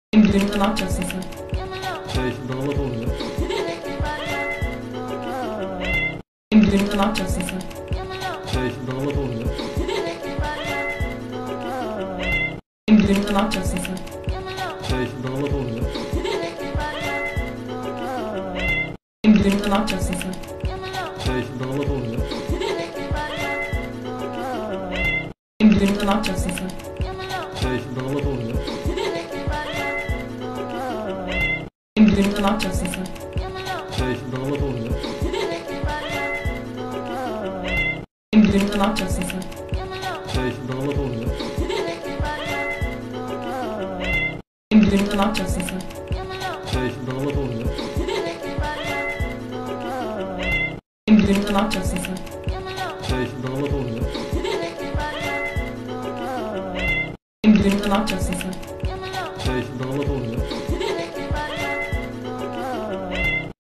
ANNA DENİZ VALESE DÜĞÜNÜMDE NE YAPACAKSIN DAMAT OLCAM DİYOR!
ANNA DENİZ VALESE DÜĞÜNÜMDE NE YAPACAKSIN VALESDE DAMAT OLCAM DİYOR!